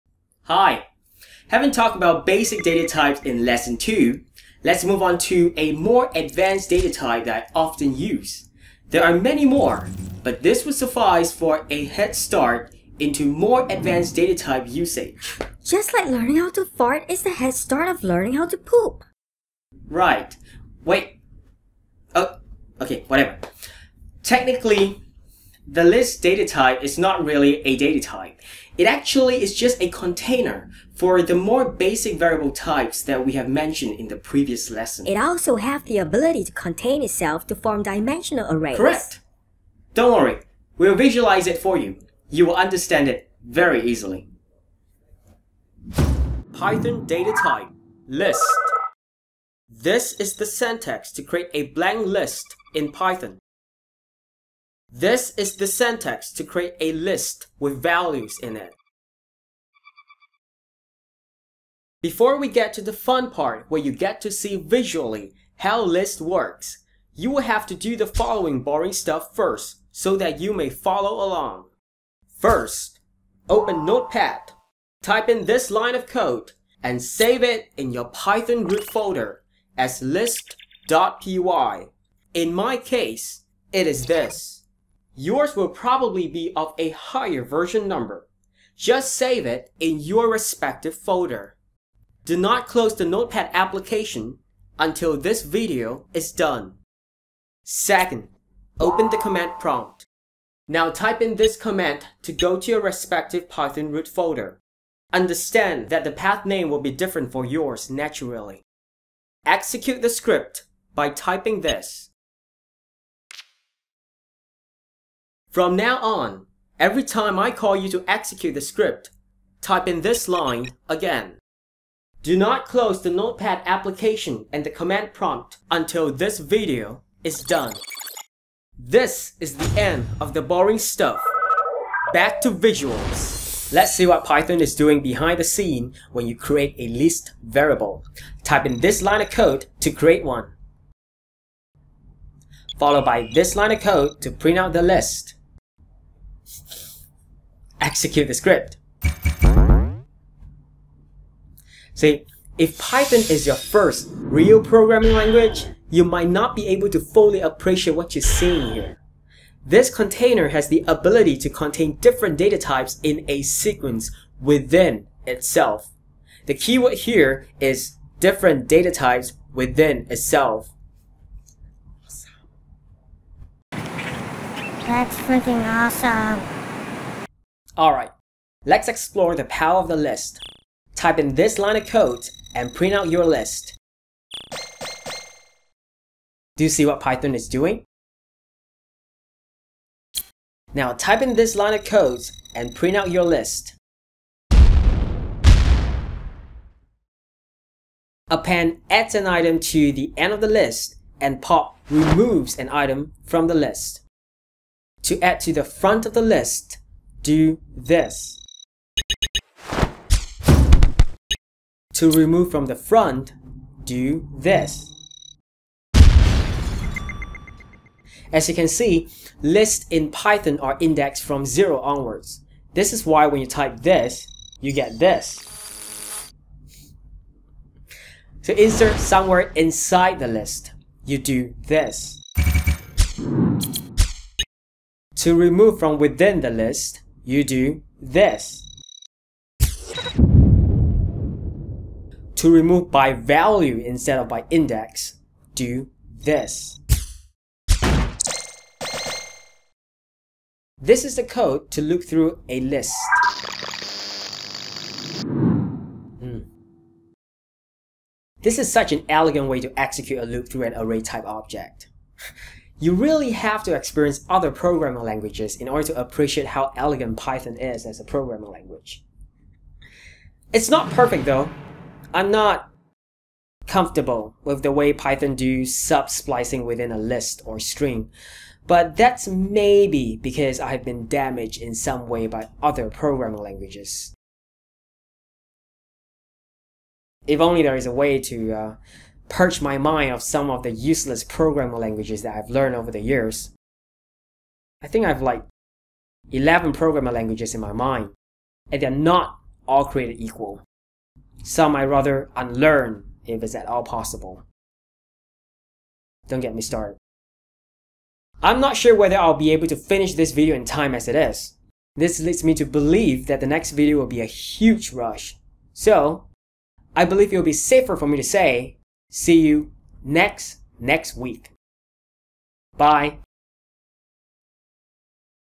Python 3 Programming for Morons 3 of 7
Part 3 of a 7 part series on Python Programming I am working on.
The next part will be out next next week :D

Kindle eBook:

Kindle eBook:

Kindle eBook:

Kindle eBook: